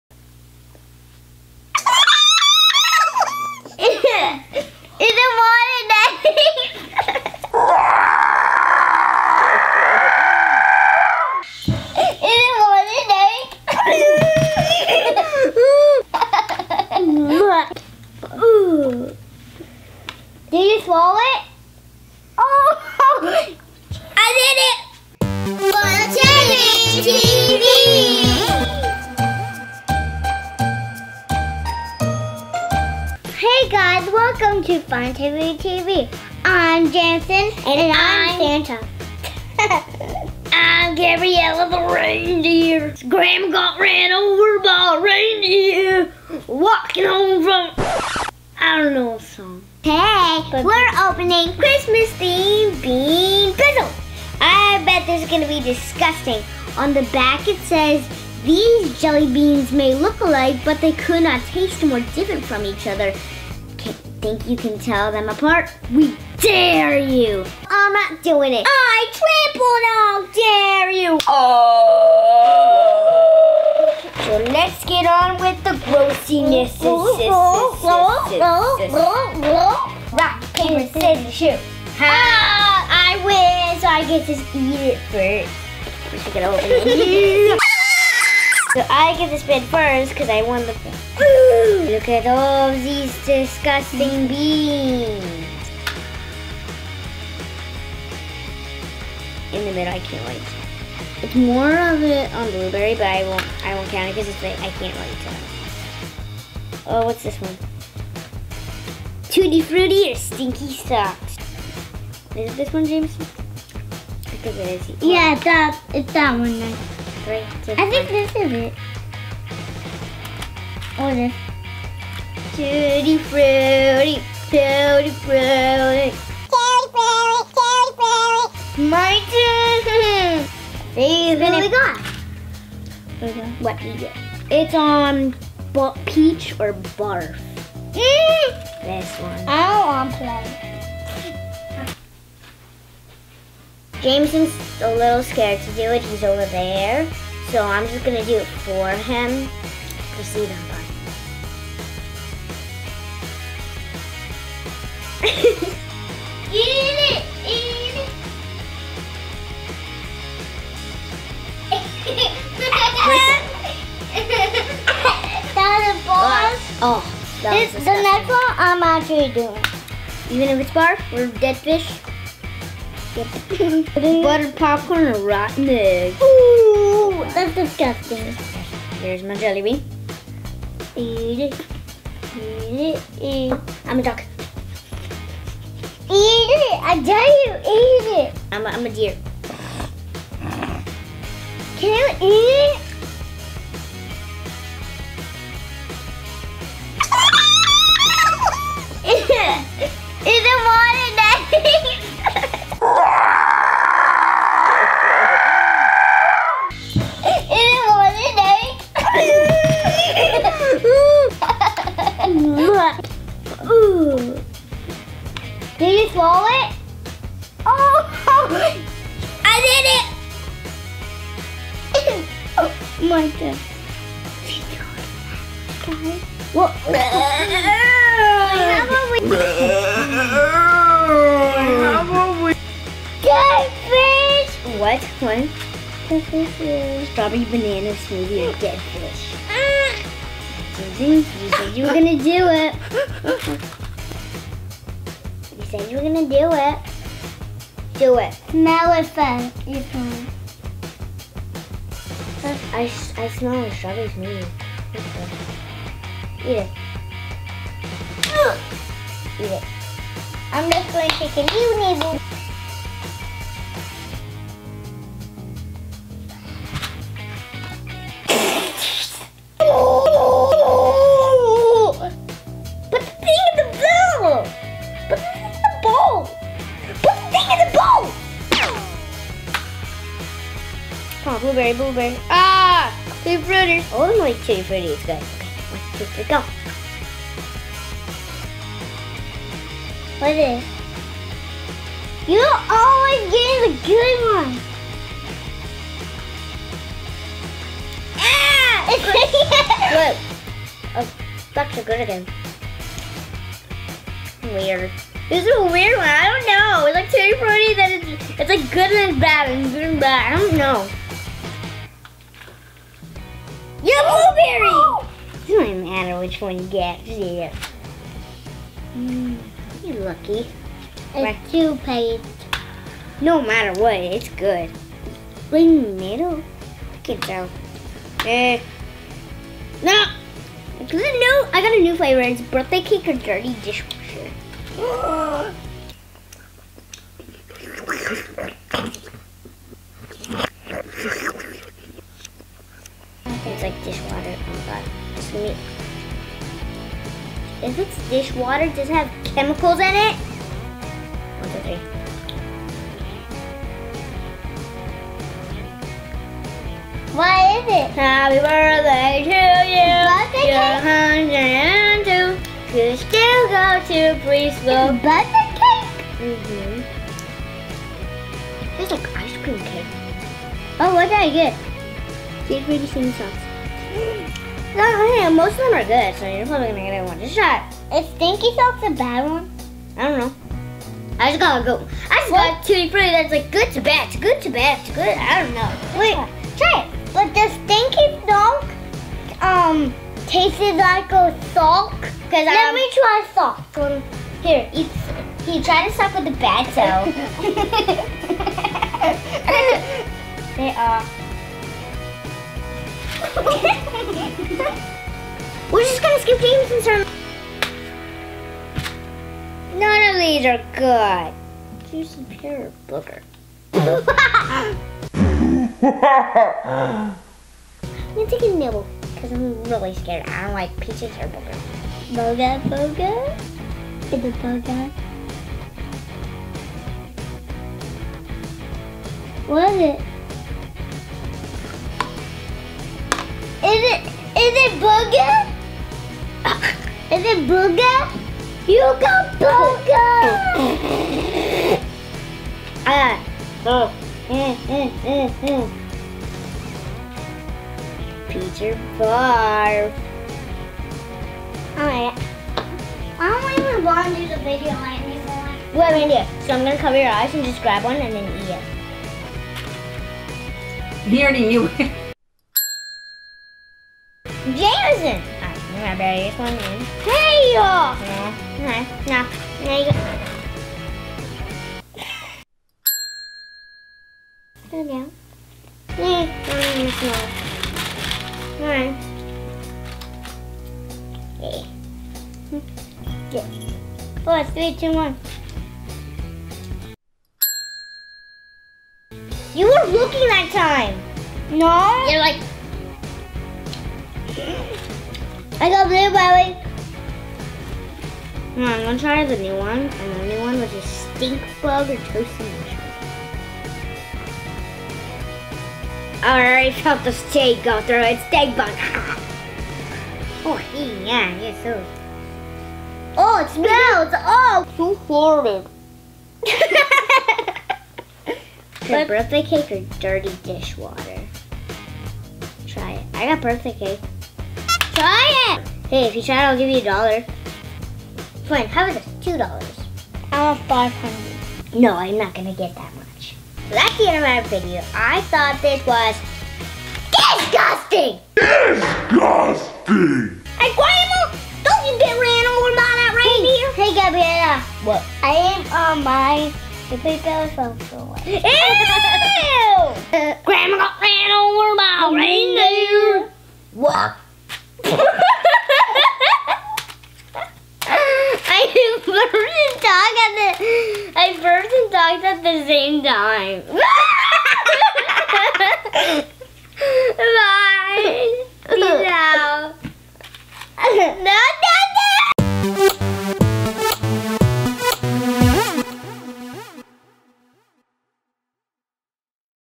CHRISTMAS BEAN BOOZLED CHALLENGE!
Gabriella and Jameson take on the "Bean Boozled" challenge with this special, "Naughty or Nice", holiday edition. How do you think they did? Watch to find out in this super gross and hilarious video!